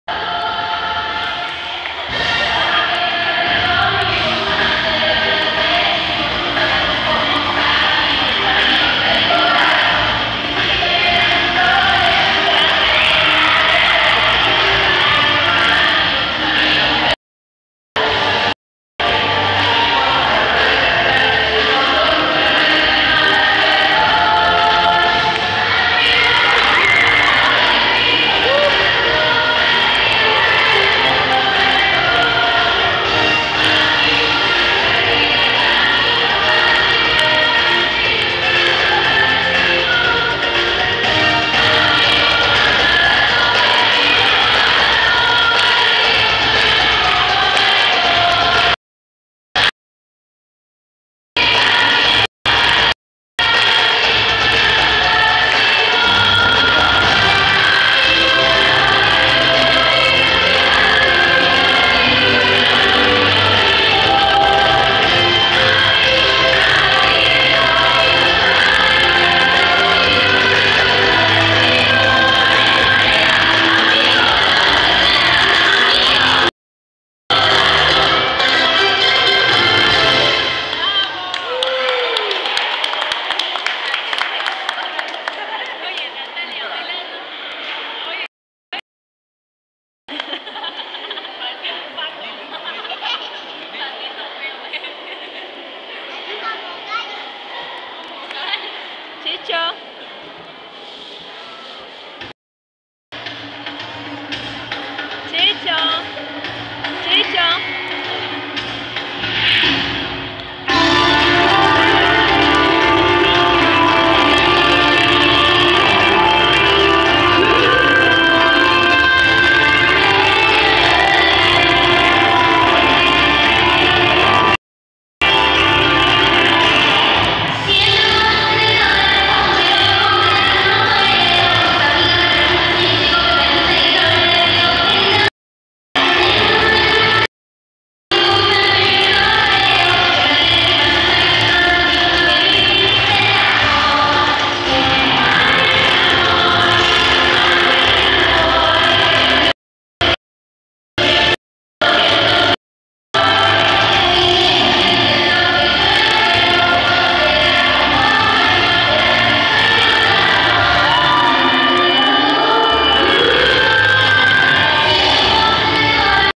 Dancing Natalia
Cotoca, Bolivia.  3 Year old Natalia Aponte is a crowd pleaser at this party at an orphanage in Cotoca.  Video shot in April 2008.